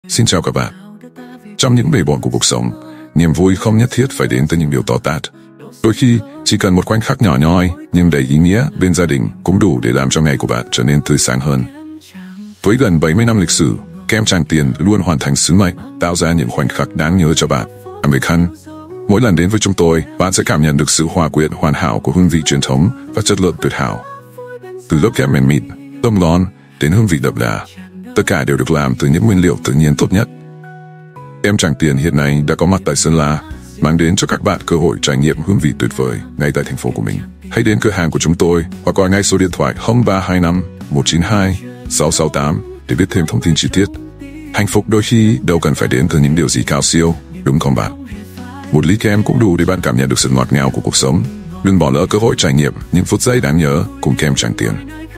Hạnh Phúc Đơn Giản Là Một Ly Kem Tràng Tiền #3
Từ tháng 4 năm 2024, thương hiệu kem Tràng Tiền nổi tiếng đã chính thức khai trương tại Sơn La. Đến ngay để thưởng thức hương vị kem thơm ngon, mát lạnh, đậm đà truyền thống của Hà Nội ngay tại quê hương mình. Đừng bỏ lỡ cơ hội trải nghiệm những ly kem chất lượng cao, được làm từ nguyên liệu tự nhiên, mang đến cảm giác sảng khoái tuyệt vời trong mùa hè này! 🍦🌞
Địa chỉ tại: Ngã tư Kem Trang Tiền – Sơn La; Điện thoại: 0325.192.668